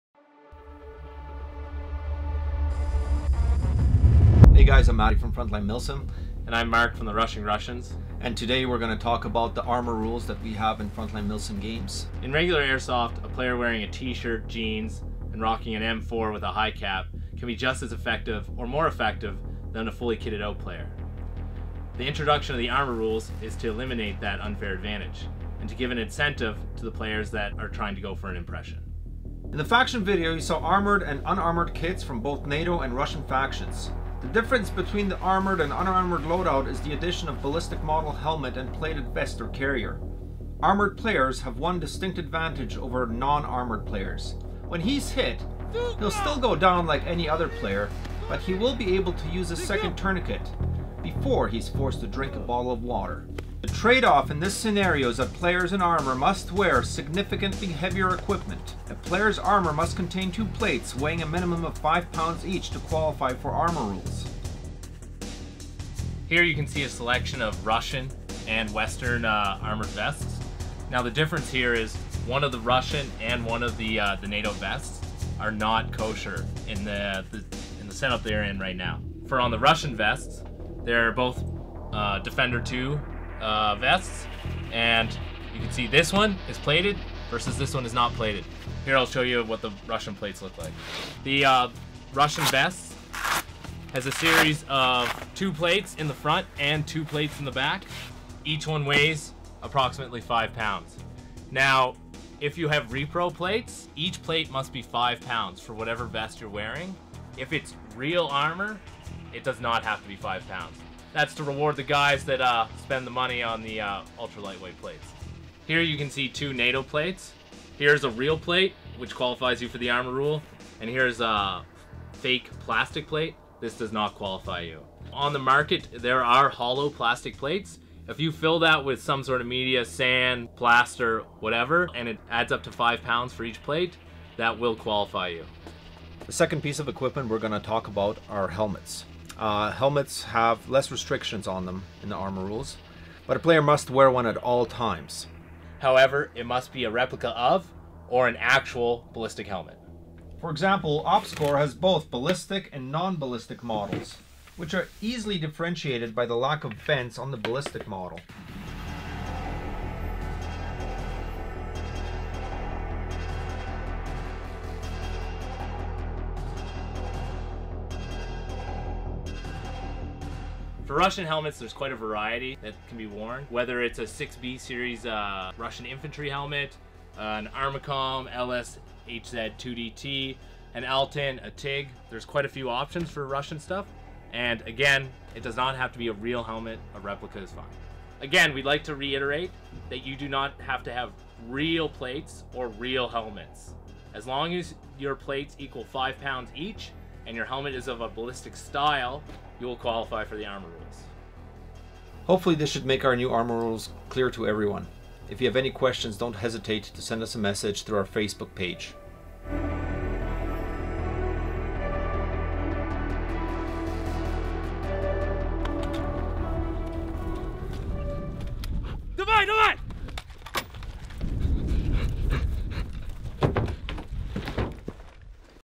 Frontline Milsim: Armour Rules
Matty from Frontline Milsim and Mark from RR NORD explain FR's armour rules, and why it's no longer silly to invest in a plate carrier for bbwars.

Armour must have:
-Minimum two plates front and back, weighing 5 pounds each
-replica or real BALLISTIC helmet (no opscore fast pj's, bj's)

If you quality for these rules, you're able to apply an extra tourniquet when shot a second time, and effectively gain an extra life. Don't forget to hydrate!